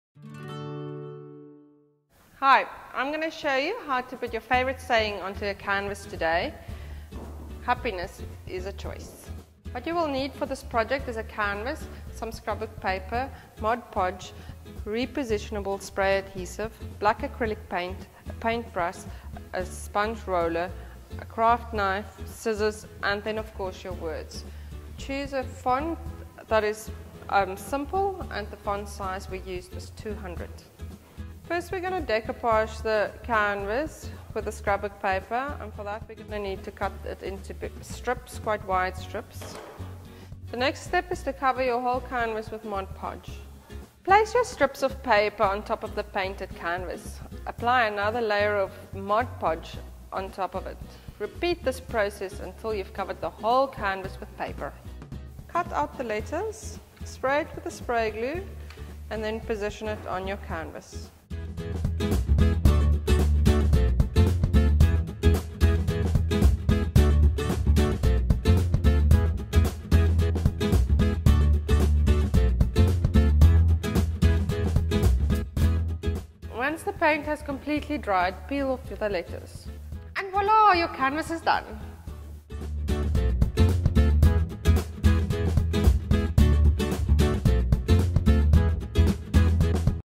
Tip: Make a canvas with your favourite quote
We all have special quotes that mean a lot to us. So let us show you how easy it is to put it onto canvas